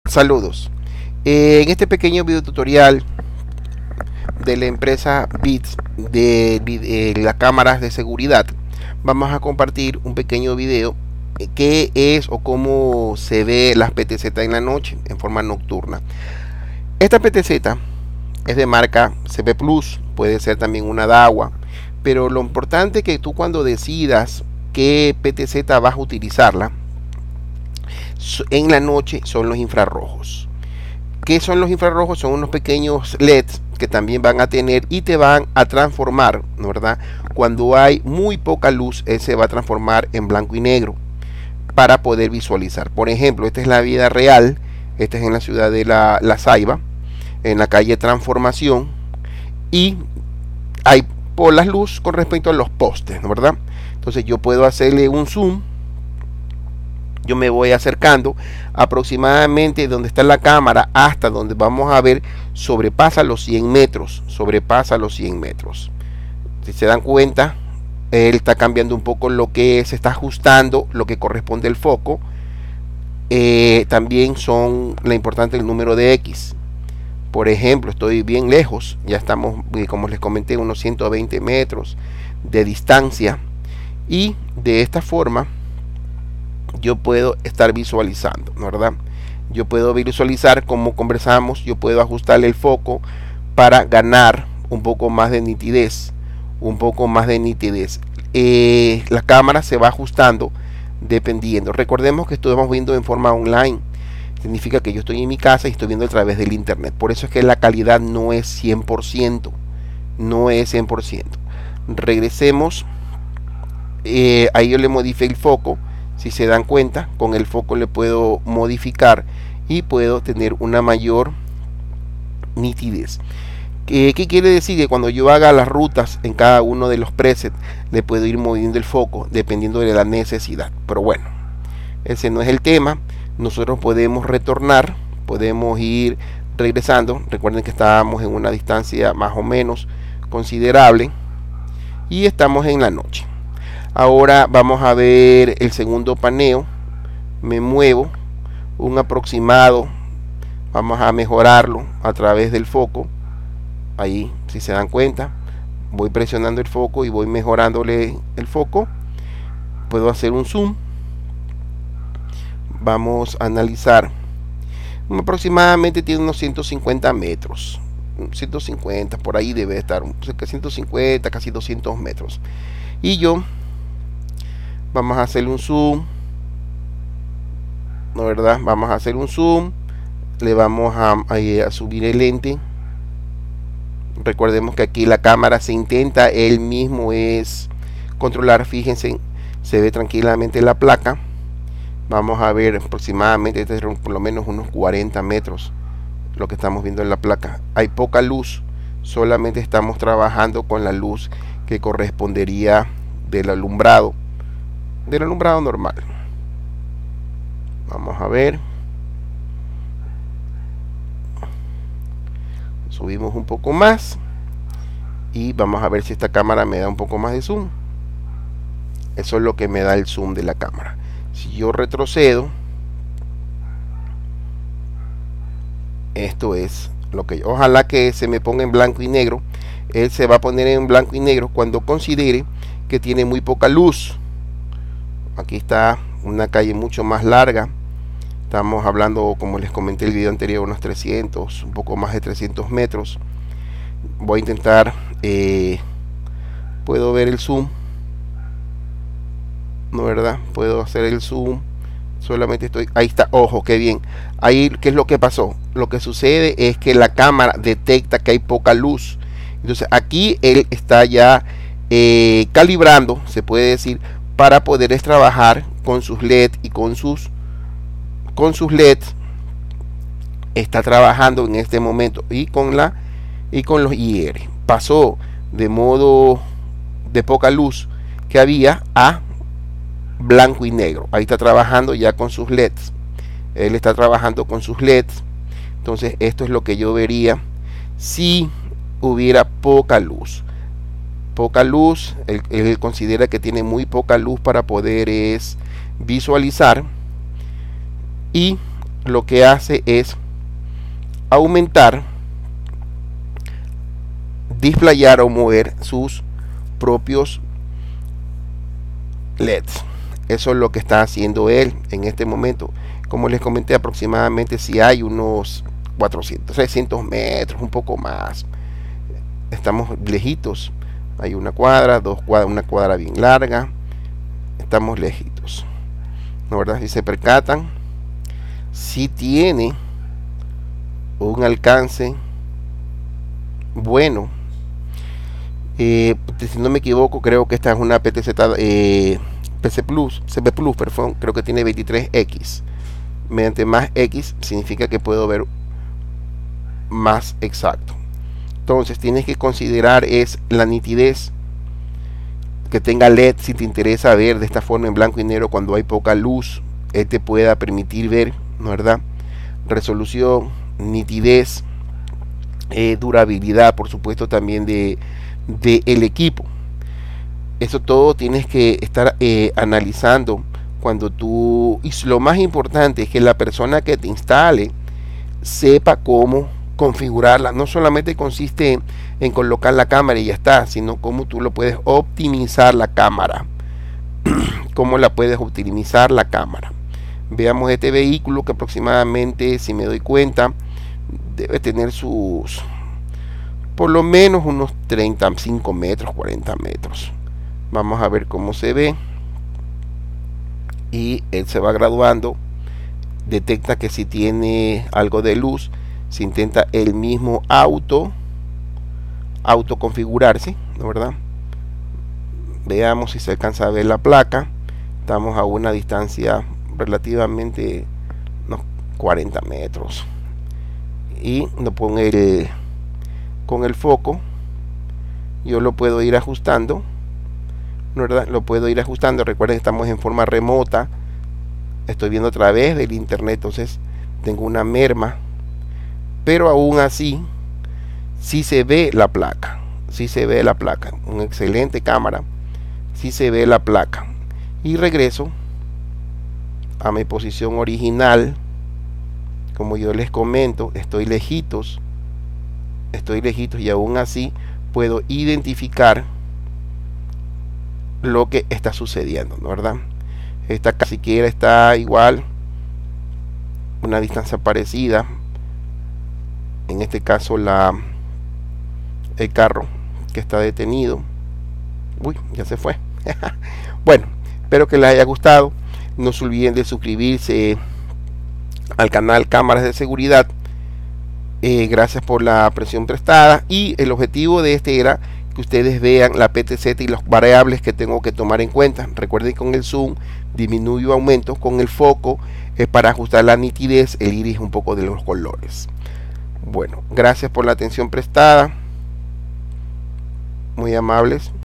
Web Service - PTZ Noche
caracteristicas de las ptz, leds, zoom, focus, etc, para que tengas una idea de lo que tienes que tomar en consideracion cuando adquieres una camara de seguridad ptz.

Nosotros instalamos, configuramos y vendemos equipos de camaras de seguridad.

Pais y Ciudad: Ecuador - Guayaquil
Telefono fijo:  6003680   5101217